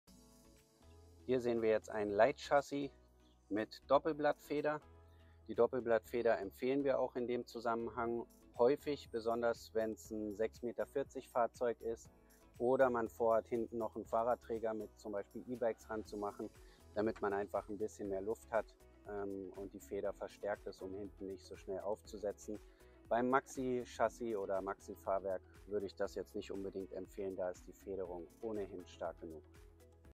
Doppelblattfeder beim Ducato oder Jumper
Empfehlenswert ist die Doppelblattfeder (17Kg) bei Light Chassis bzw. Light Fahrwerk, wenn man entweder eine Fahrzeuglänge von 6,40 m gewählt hat, oder viel Gewicht auf der Hinterachse hat. Beispielsweise durch einen Fahrradträger oder eine Box für die Anhängerkupplung. Beim Maxi Fahrwerk/ Heavy Chassis würde ich es zum einen aus Gewichtsgründen nicht empfehlen und zum anderen ist die Federung dieses Fahrwerks ohnehin schon stärker.